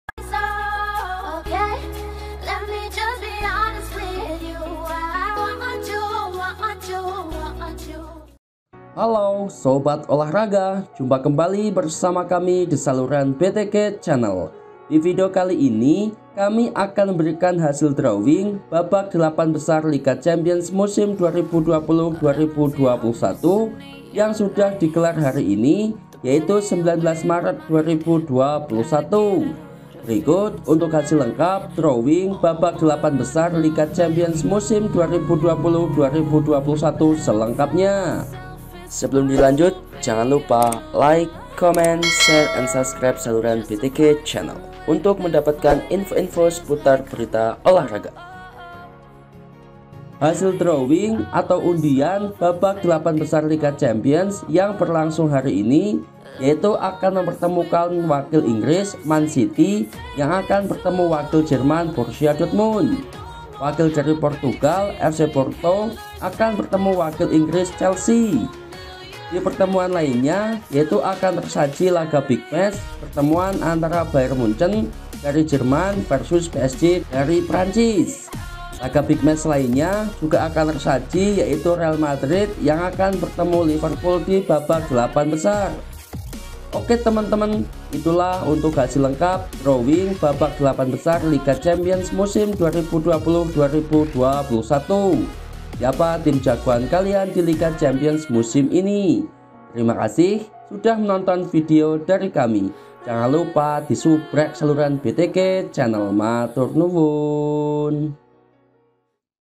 Hasil Drawing 8 Besar Liga Champions 2021: Madrid vs Liverpool, B Munchen vs PSG | UCL Round OF 8
Hasil Drawing 8 Besar Liga Champions2021: Madrid vs Liverpool, B Munchen vs PSG | UCL

Berikut hasil drawing babak perempat final atau babak 8 besar liga champions 2021 yang sudah di gelar hari ini. akan ada pertemuan big match yaitu antara Real Madrid vs Liverpool dan Bayern Munchen vs PSG di babak 8 besar atau perempat final liga champions 2020/2021

Berikut hasil lengkap drawing atau undian babak perempat final liga champions.
MAN CITY VS B DORTMUND
FC PORTO VS CHELSEA
BAYERN MUNCHEN VS PSG
REAL MADRID VS LIVERPOOL

TERIMA KASIH.

Berita Olah Raga
Hasil Bola
Hasil Bola Terbaru
Jadwal Bola
Jadwal Bola Terbaru
Hasil Bola 2020
Jadwal Bola 2020
hasil bola terbaru 2020